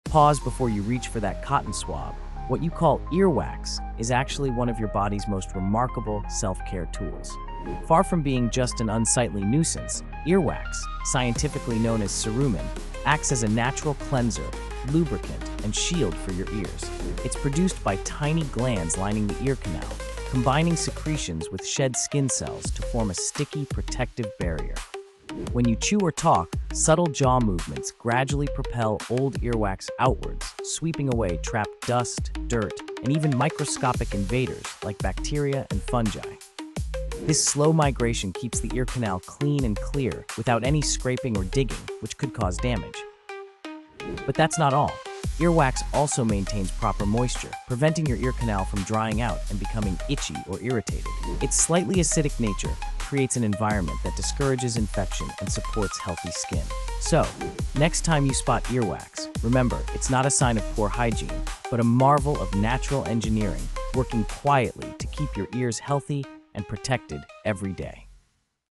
Earwax: Nature’s Ingenious Ear Defender
Think earwax is just gunk? Think again! Discover how this natural marvel protects, cleans, and keeps your ears healthy.